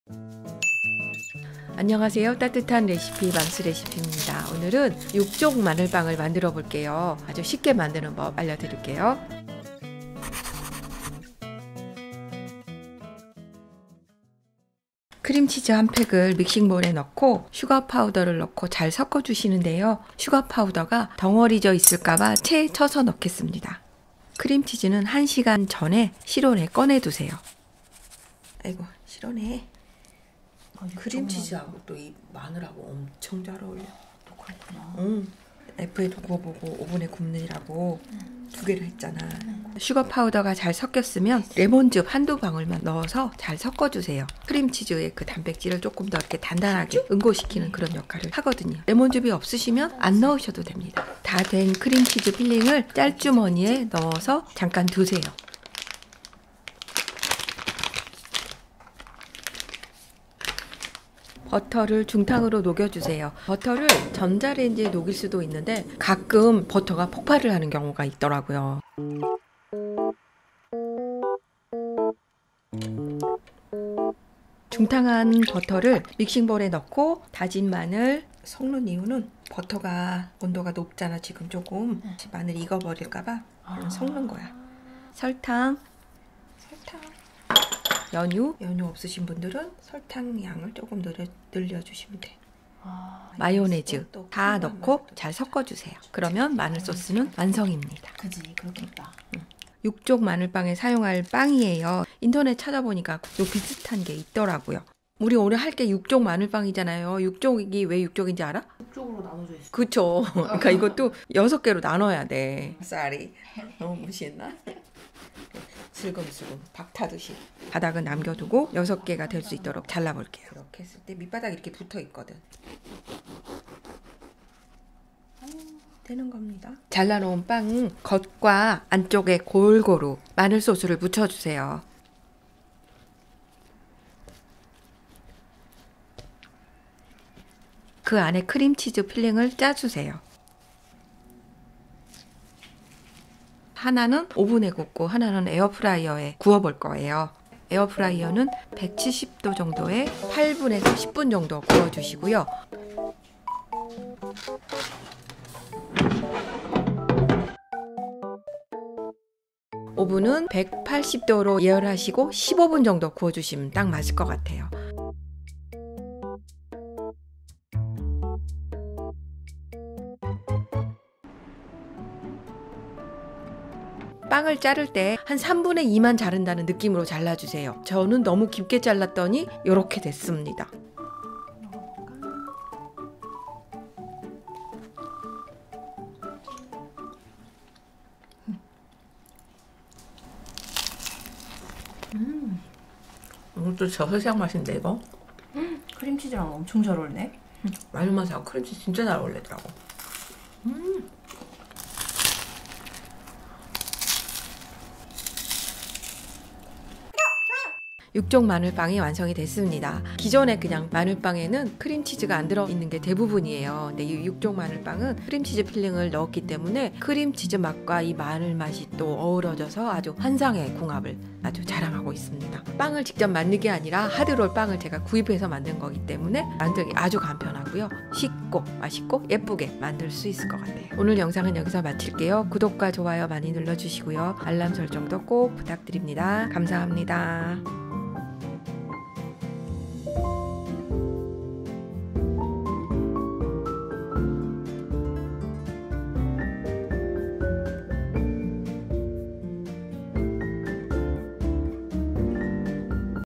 에어프라이어&오븐으로 육쪽 마늘빵 만들기 | 3개 3천원 하드롤로 만들어봄!
오늘은 저렴한 하드롤을 이용해서, 육쪽마늘빵을 만들어볼게요 :)
인터넷에서 쉽게 구할 수 있는 하드롤!!
크림치즈를 곁들인 육쪽마늘빵 소스로 맛있는 육쪽마늘빵 만들어봤어요.

더 좋은 영상을 만들 힘이 됩니다♥
궁금하신 레시피가 있으시다면 댓글로 남겨주세요!

*재료(밥숟가락 계량)
·하드롤 2개 분량
·버터 90g
·마요네즈 20g
·연유 20g
·다진 마늘 30g
·설탕 10g
·파슬리가루 약간
·크림치즈 200g
·슈거파우더 80g
·레몬즙 약간